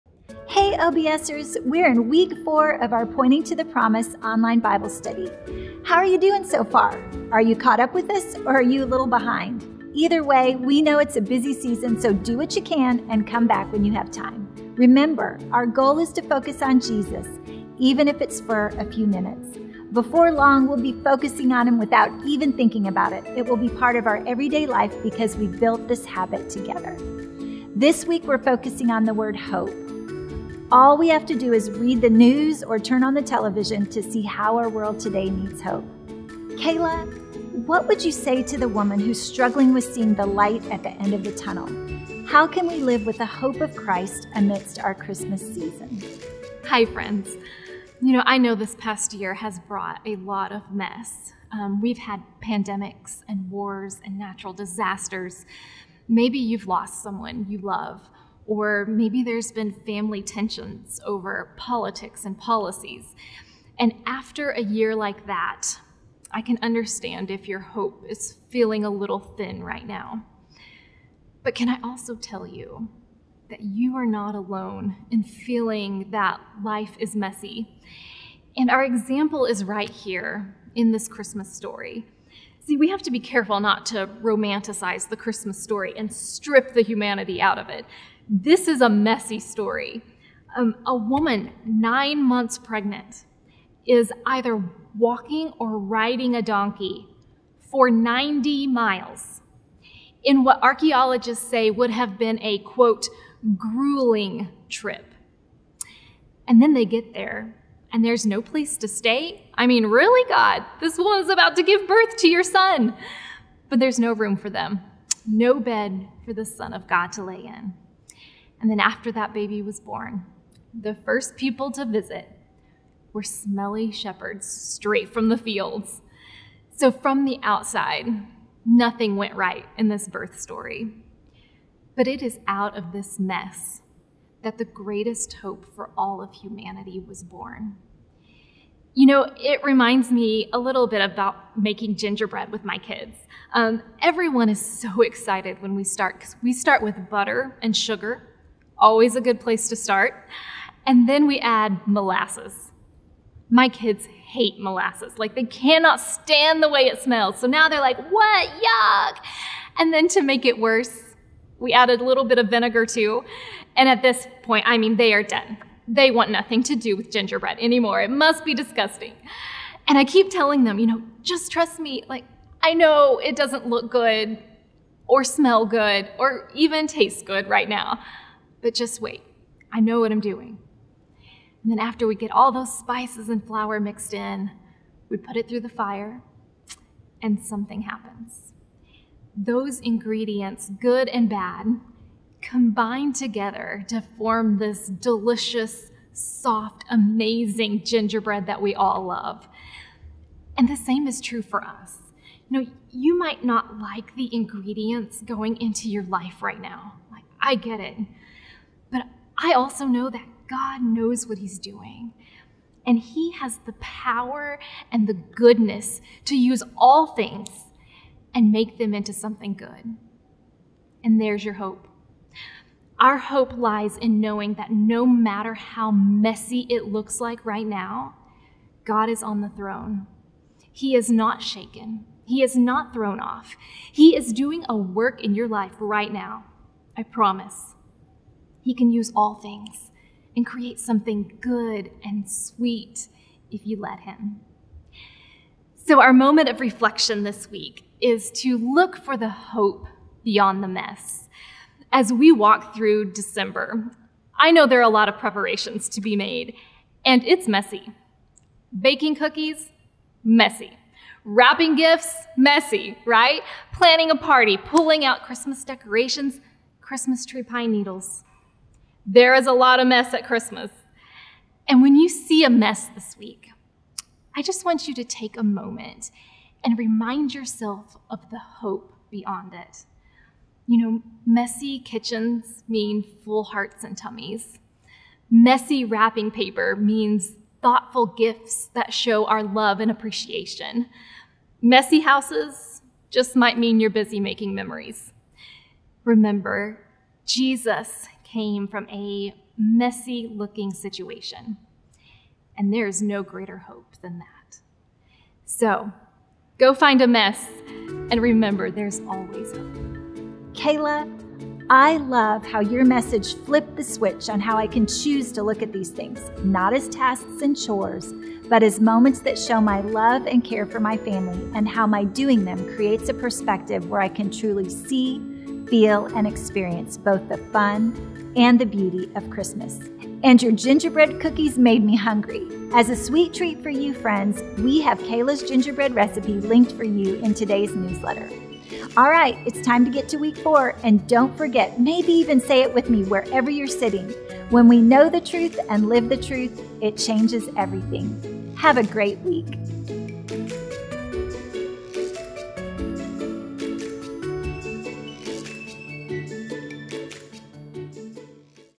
The Light at the End of the Tunnel | Pointing to the Promise | Kayla Ferris | OBS Week 4
No matter what it looks like around you, God is in control. We know that this past year has brought a lot of pain. Maybe you have lost people you love or maybe there have been family tensions over politics and policies. After a year like that, we understand if your hope is feeling a little thin right now. But you are not alone in feeling that life is messy. And our example is right here in this Christmas story.

Give God the first 5 minutes of your day with the FREE First 5 app